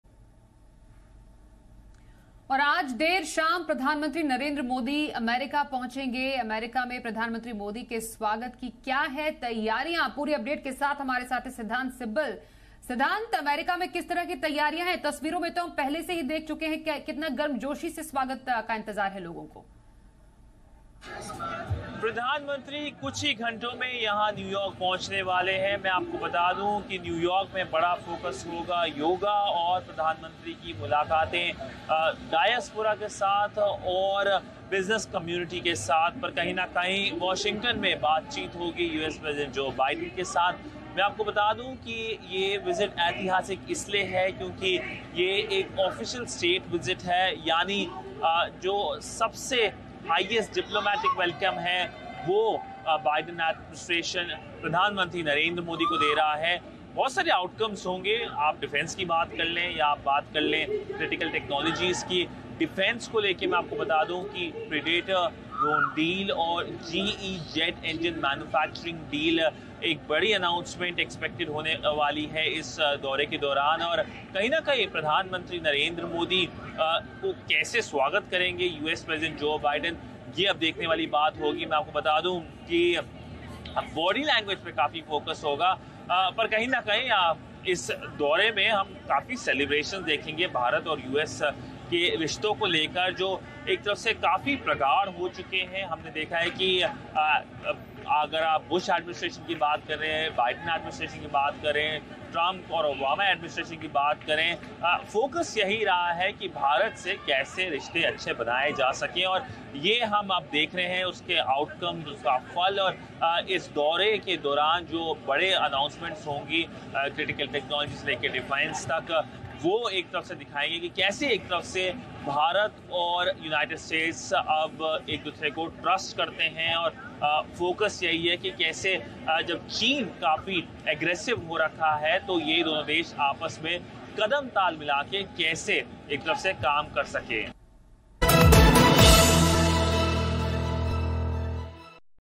PM Modi US Visit: From Yoga day celebrations to USA Congress address
PMmodi अमेरिका की अपनी आधिकारिक यात्रा के दौरान न्यूयॉर्क में स्थित संयुक्त राष्ट्र मुख्यालय में 21 जून को InternationalYogaDay2023 के 9वें संस्करण का नेतृत्व करेंगे.